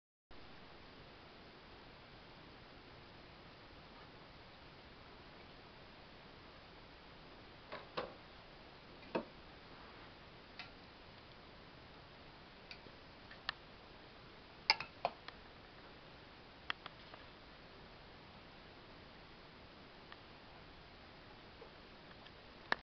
Scorpion eating Krill
Me feeding the new guy. He started eating frozen on Day 1, and hasn't looked back.